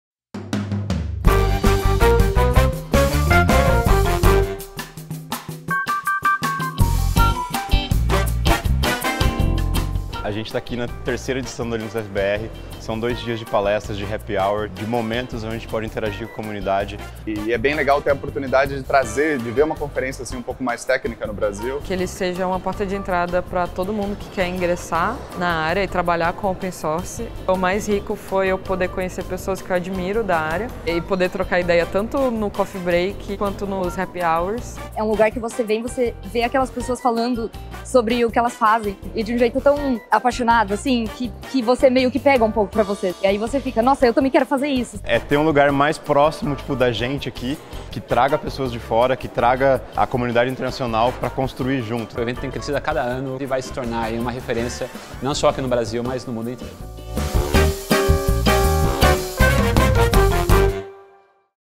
linuxdev-br 2019 - Destaques
Linux Developer Conference Brazil aims to take the Brazilian Linux development community to the international level. Whether you are just curious and want to understand the Linux ecosystem, someone seeking to contribute to FOSS projects, or even a seasoned collaborator, this conference is for you.